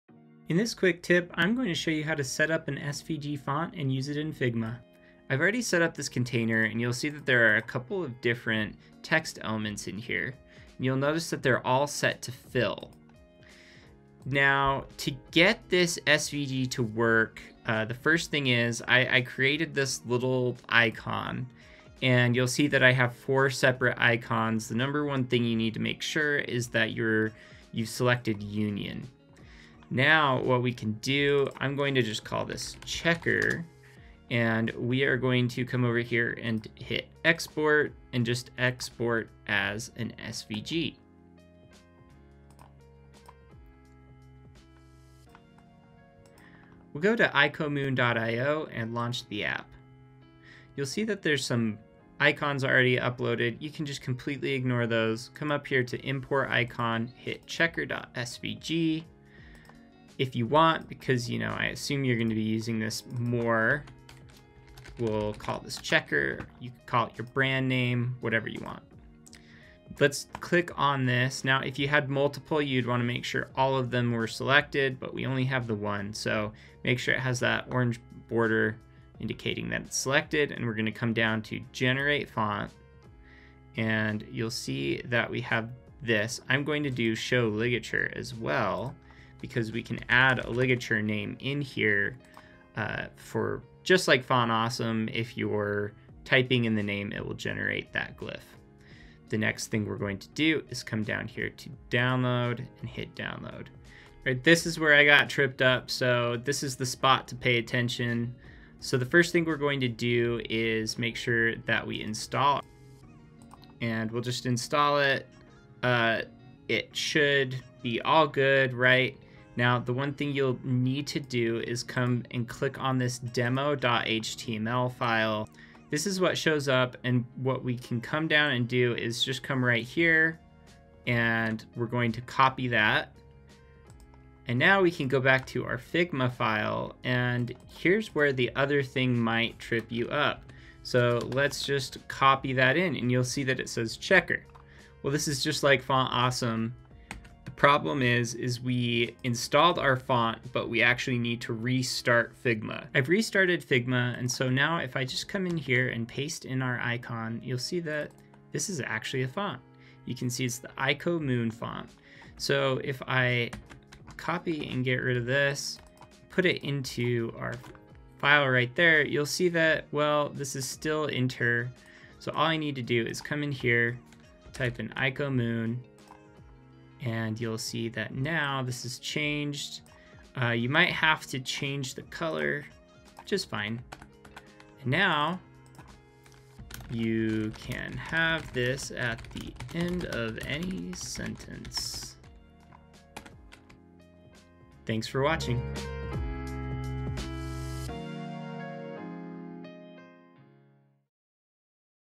Convert SVG to fonts to create an icon library in Figma.
Learn how to use Figma to export your SVG files into icomoon.io. Working from SVG to TTF files.